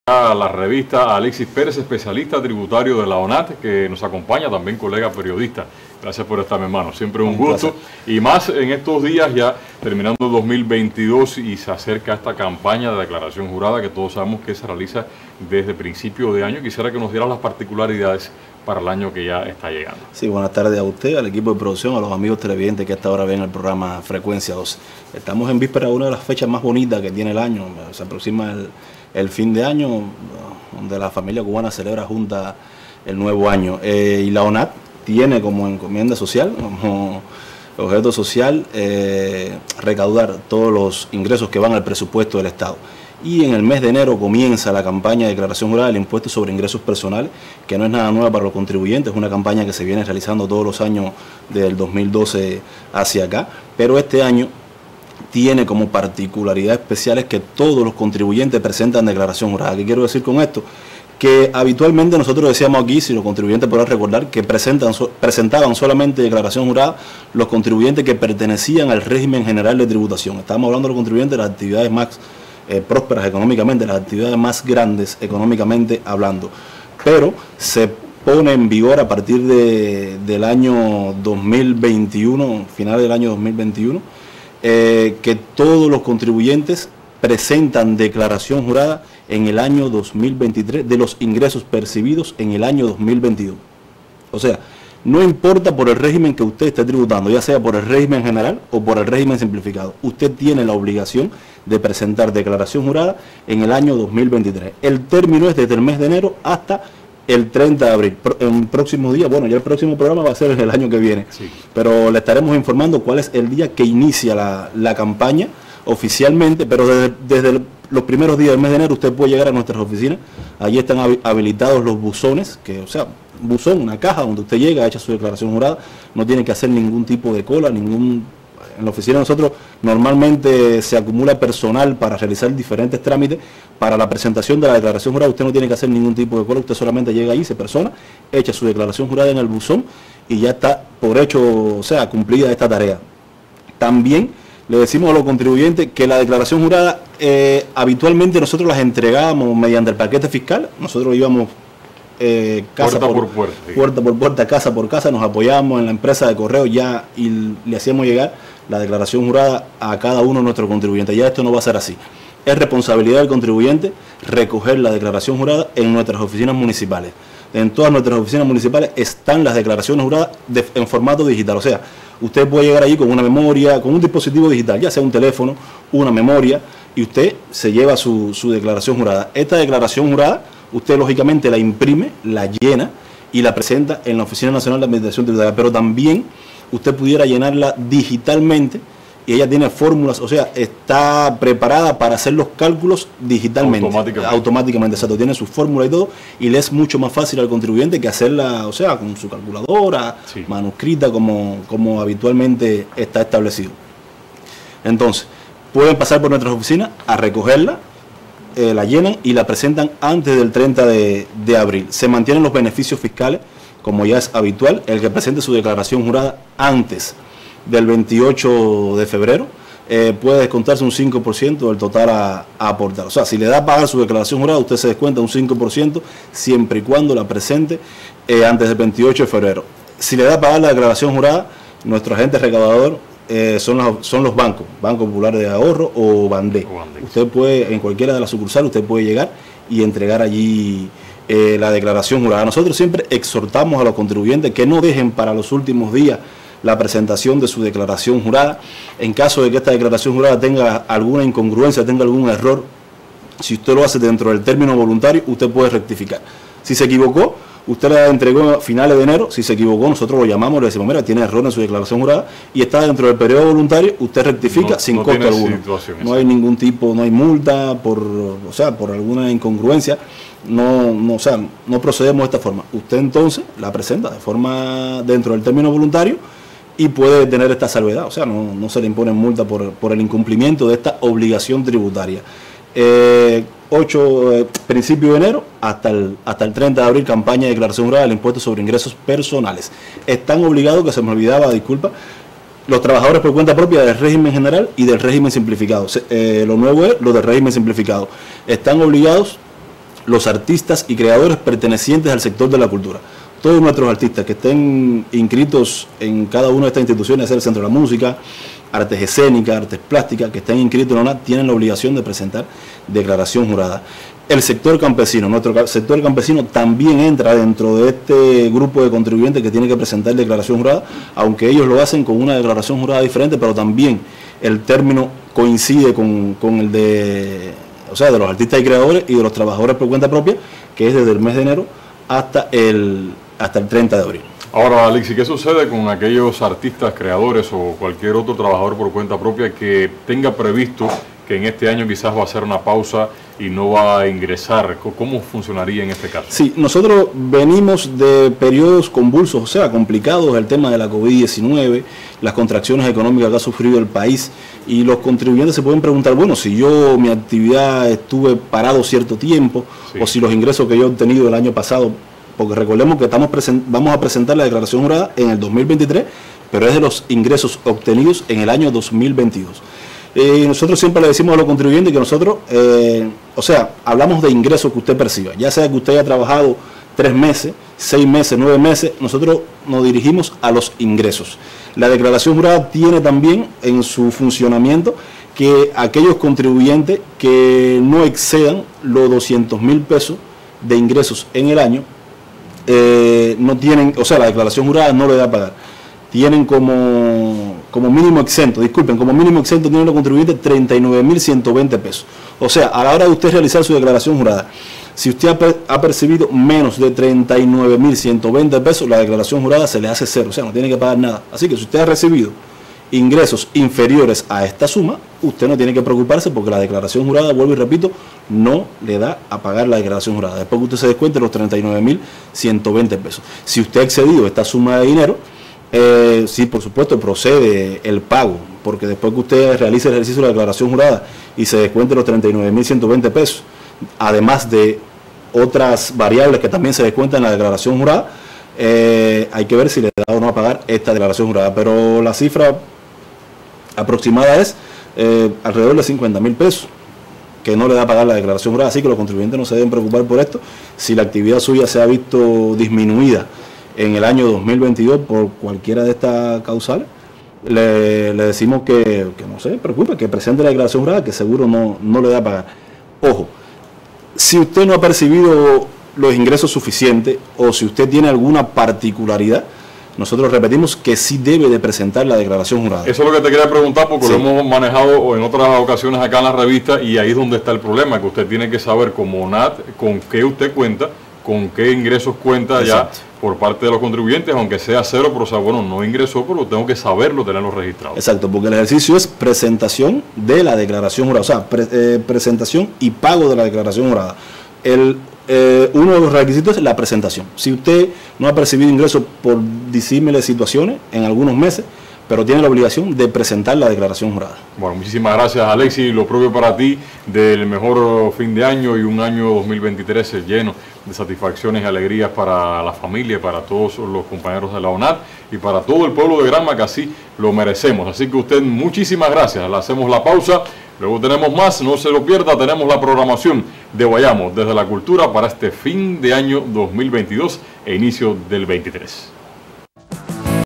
Precisan nuevos detalles sobre la Campaña de Declaración Jurada sobre los ingresos personales, 2023
A partir del próximo mes de enero da inicio a la Campaña de Declaración Jurada, donde todos los contribuyentes presentarán declaración de los ingresos recibidos, estos y otros detalles los brinda el especialista tributario Alexis Pérez Ramírez, durante su intervención en el programa Frecuencia 12.